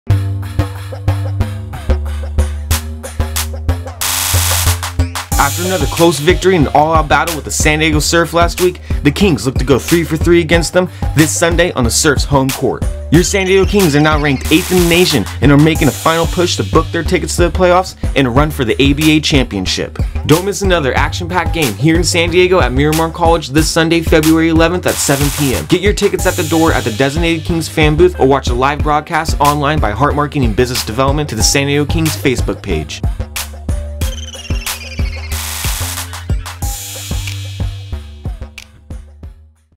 Kings VS Surf VOL 2 4k
Don't miss out on this Sundays LAST San Diego battle of the season between your San Diego Kings Basketball and the SD Surf 7pm at Miramar College.